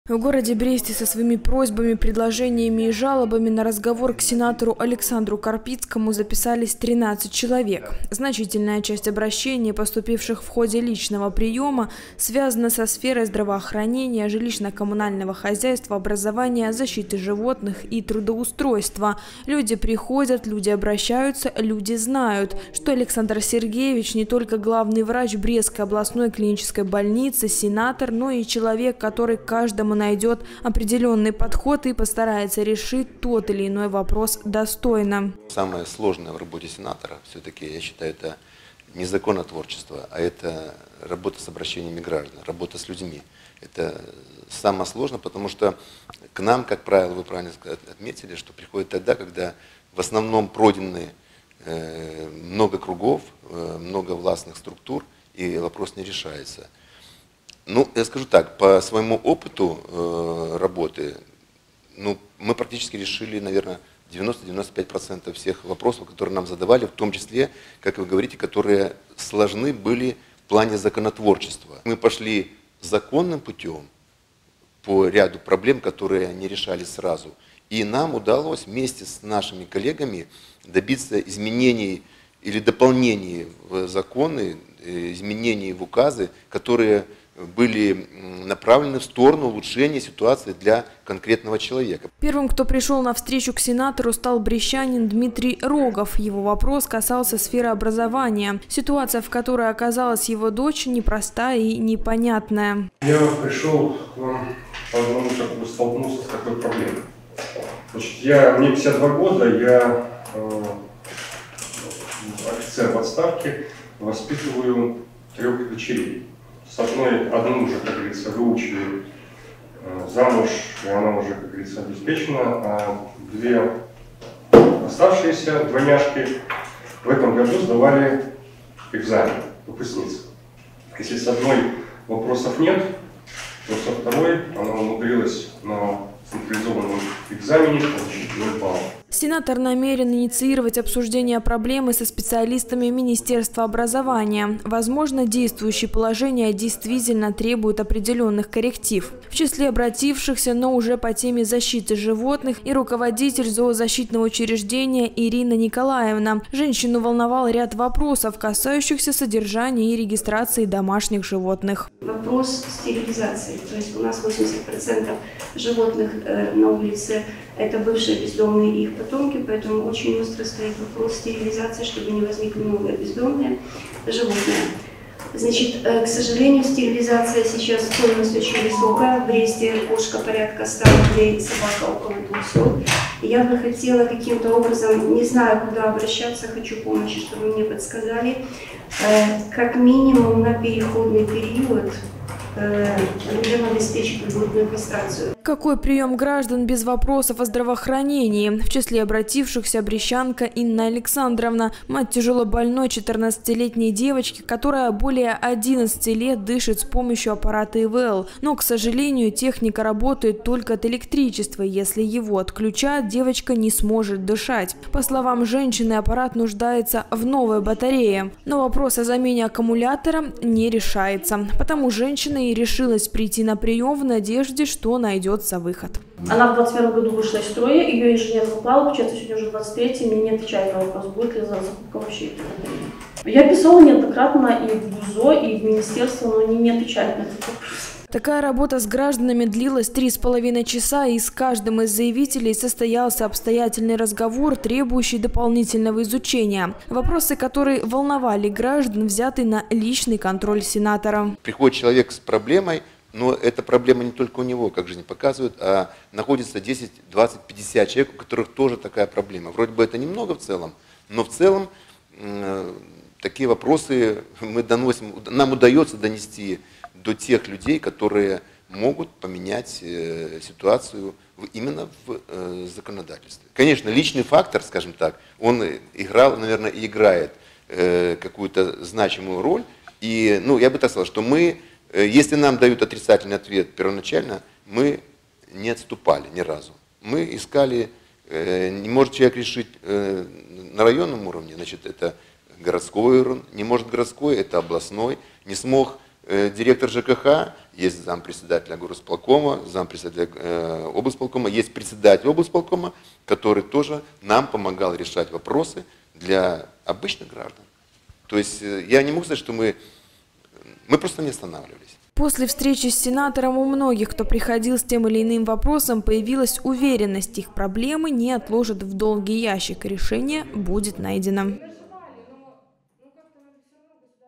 2023-07-20 Сенатор А. Карпицкий провел прием граждан
Единый день приема граждан накануне провели члены Совета Республики Национального собрания. В этот раз парламентарии посетили все районы Брестчины и встретились с жителями, чтобы оказать содействие в разрешении волнующих вопросов. Член Постоянной комиссии Совета Республики по международным делам и национальной безопасности Александр Карпицкий провел прием граждан на базе Брестской областной клинической больнице. С какими просьбами, предложениями и жалобами обращались граждане, - расскажет Анастасия Мелишкевич
*Наши контакты:*
✅Телефон горячей линии:
54-63-67